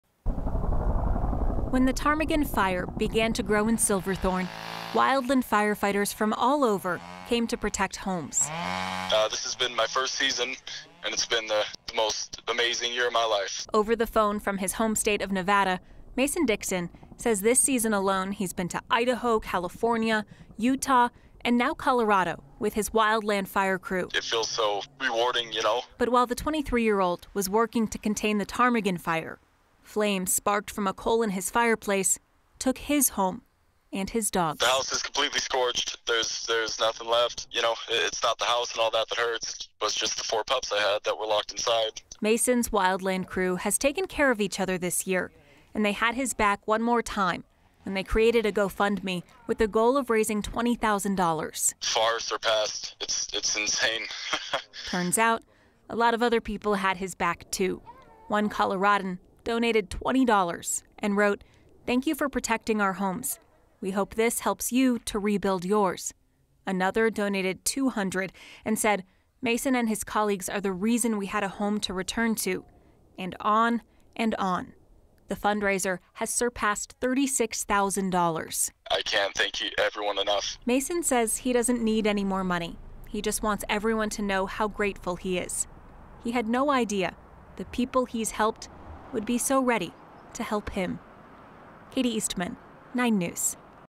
Ptarmigan firefighter loses Nevada home in house fire; community reaches out to help
One firefighter had to leave Colorado this week - because of a fire at his own home.